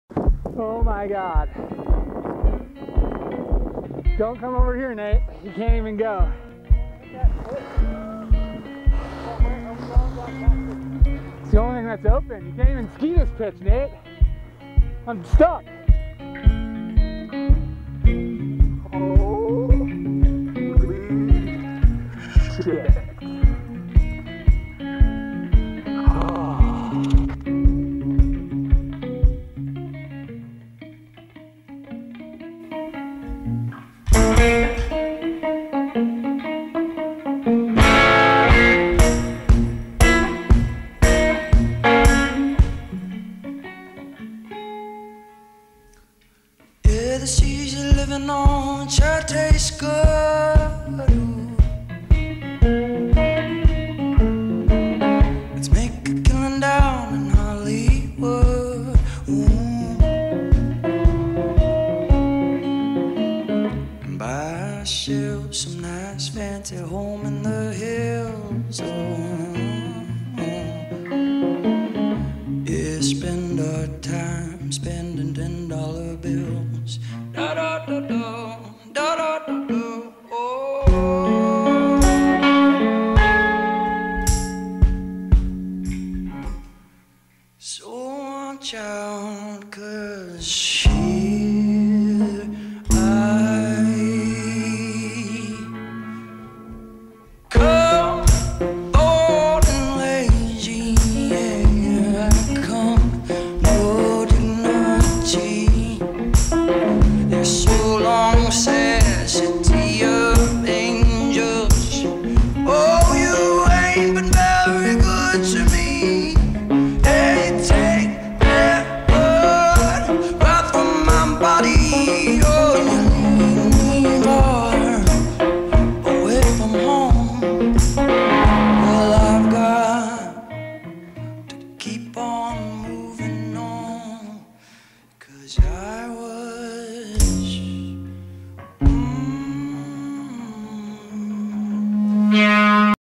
2 12 2019 Crystal Mountain
One of the deepest day of the decade. Heavy and challenging, but still fun.